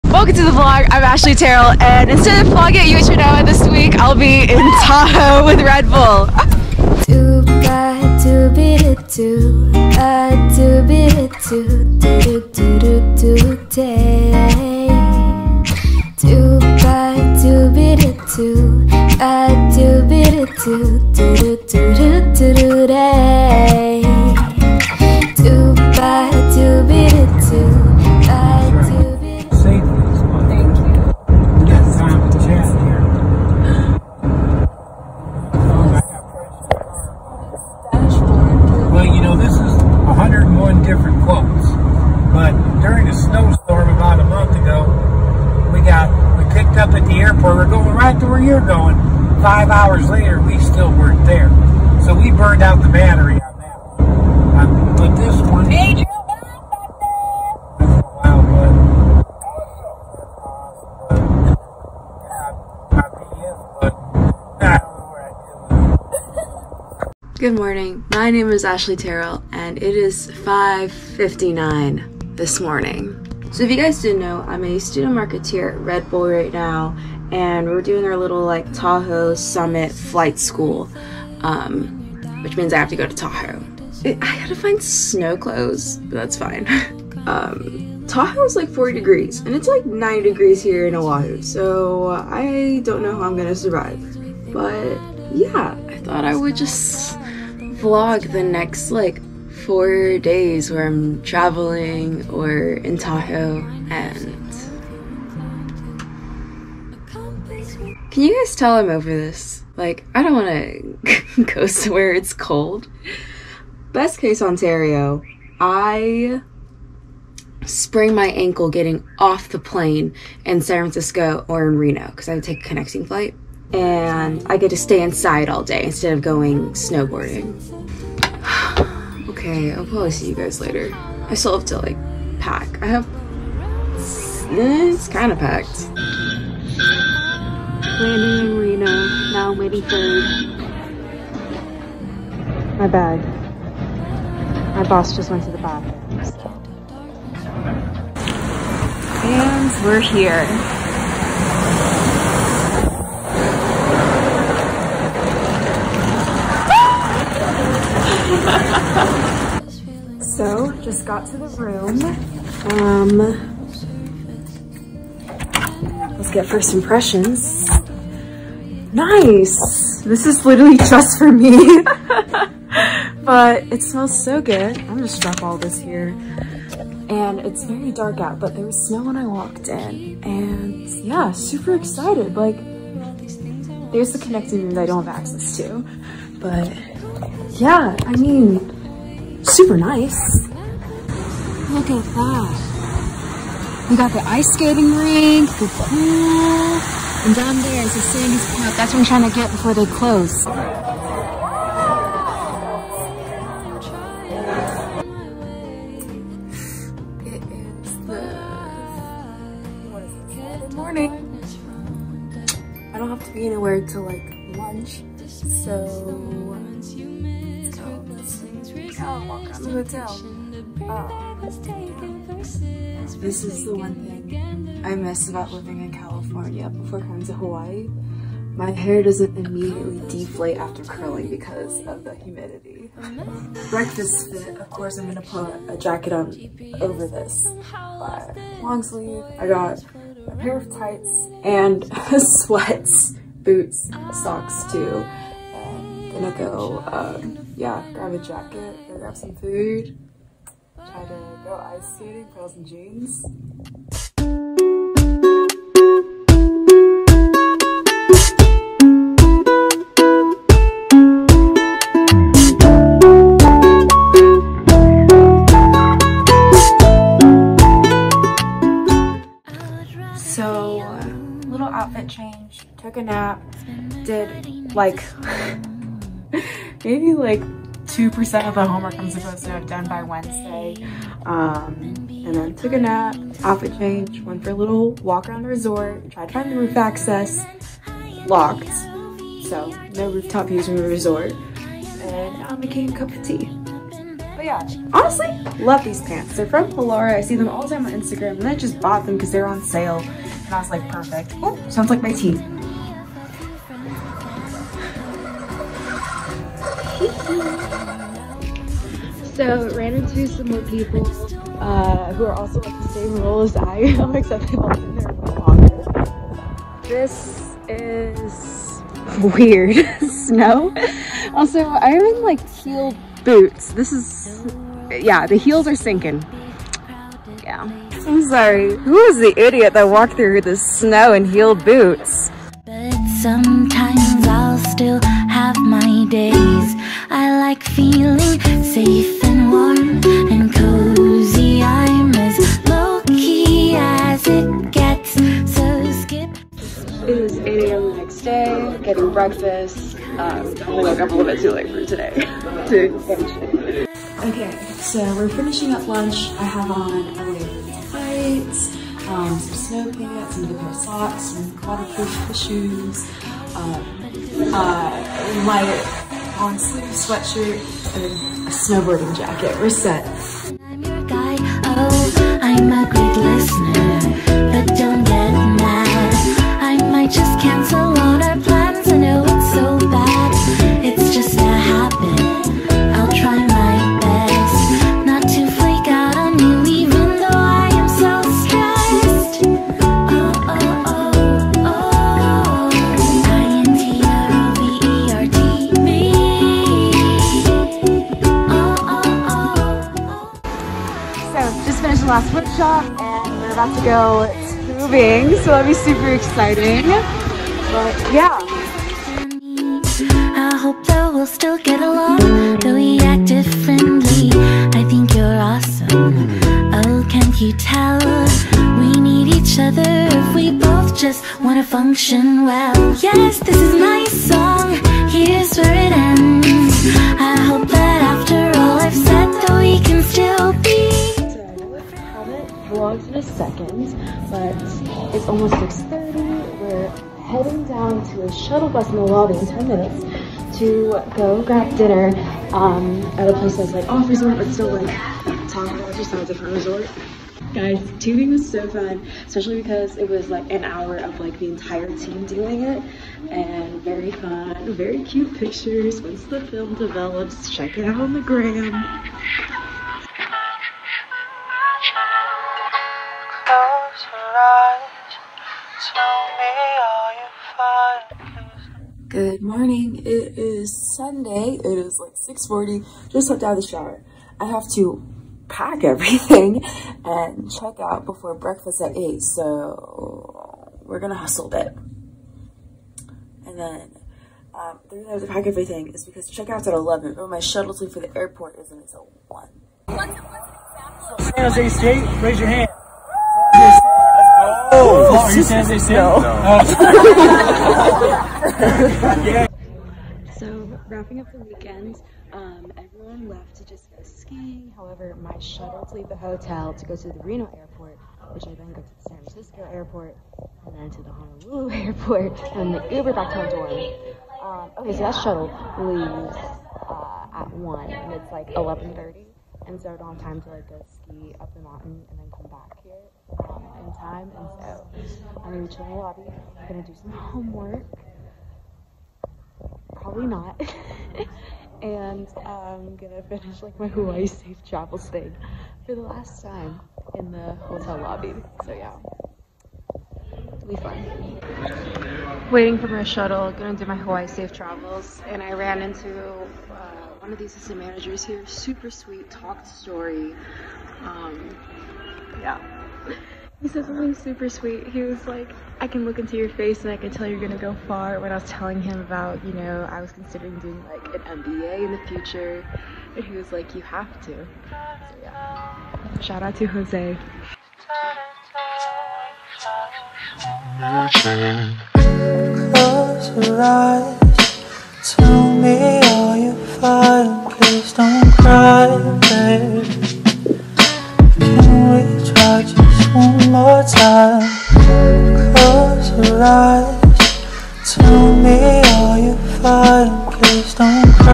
spring break w/ Red Bull (UH Manoa vlog)
tysm red bull! so amazing to meet and tube with everyone!